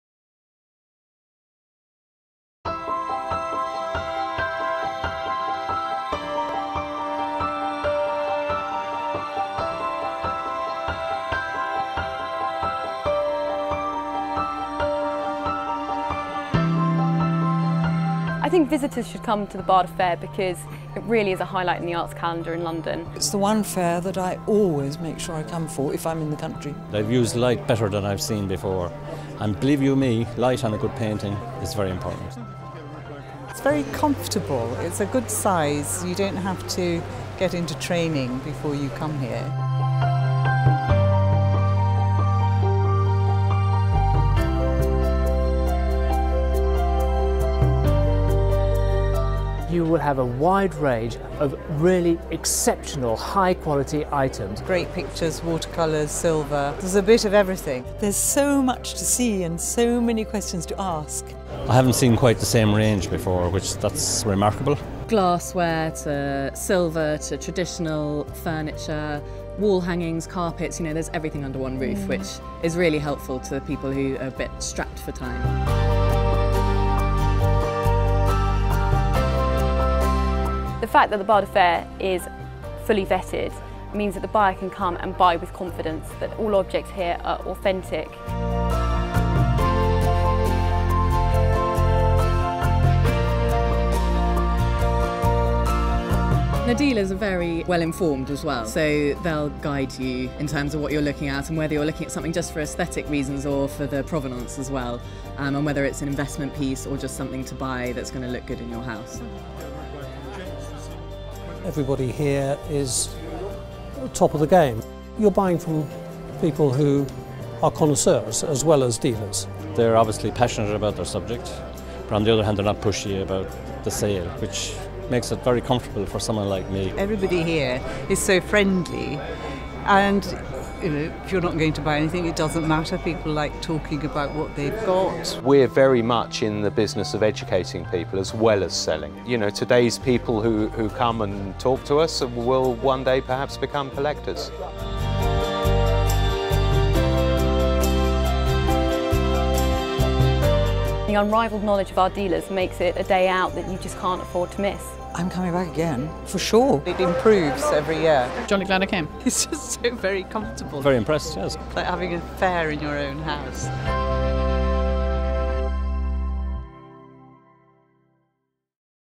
BADA 2016
For over 20 years the annual BADA Fair has been the spring highlight of the fine art, design and antiques calendar; consistently showcasing pieces of outstanding quality from 100 of the UK’s finest dealers.

Held on Chelsea’s Kings Road, the week-long event attracts collectors, curators, art professionals and interior designers from around the world.

BADA Fair 2017, 15 - 21 March